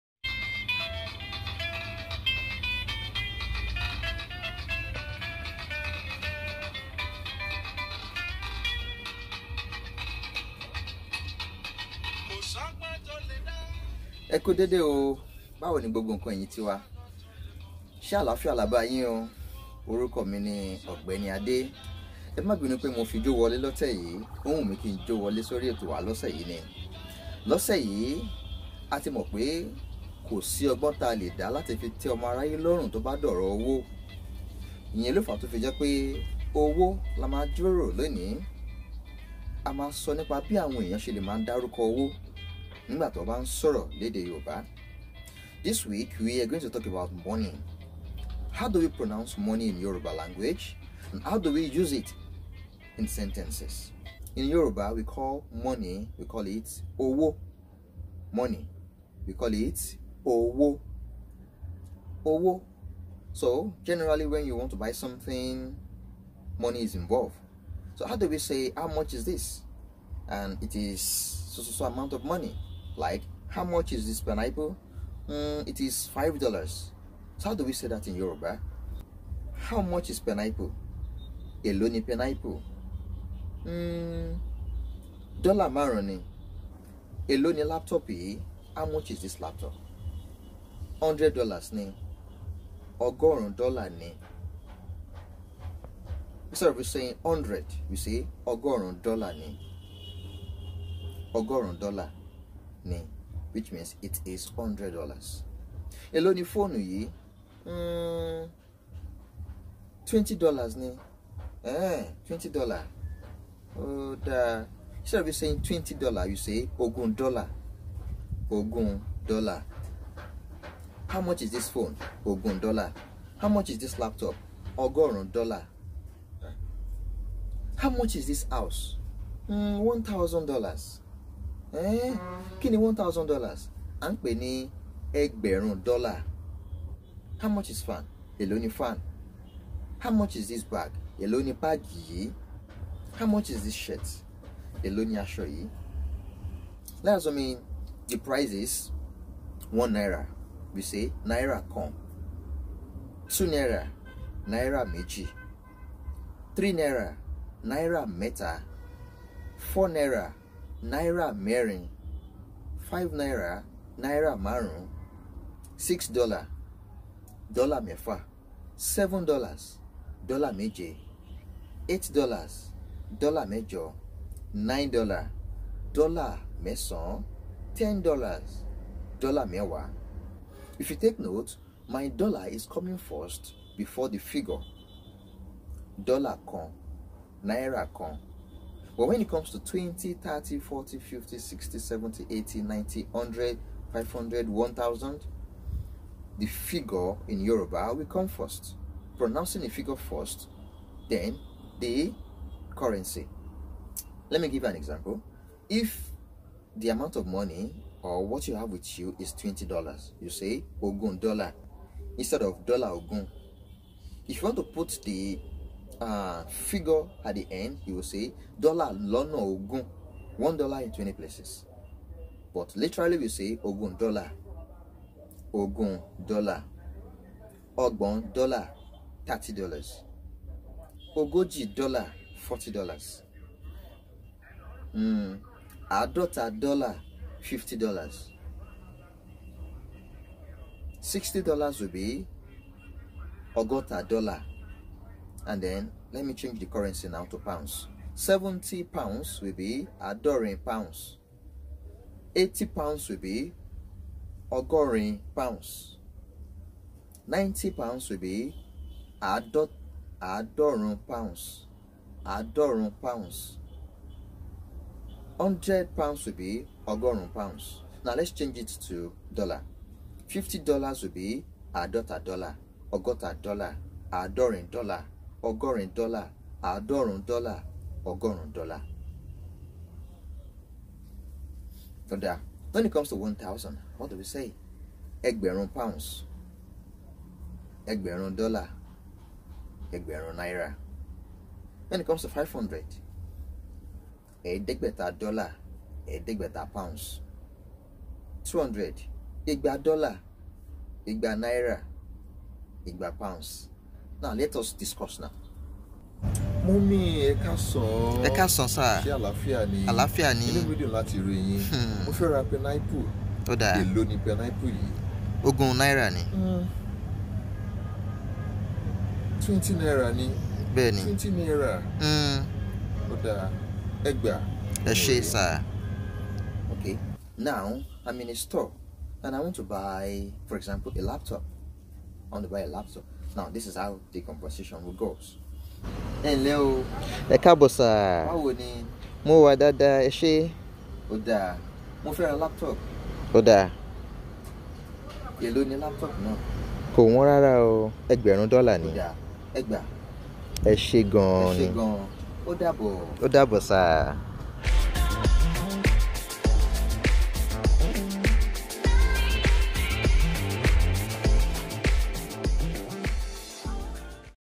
Owó-Money - Using Money in Yorùbá Sentences. Ogbeni Ade
In this video. I explained what owó is and how to use it in sentences.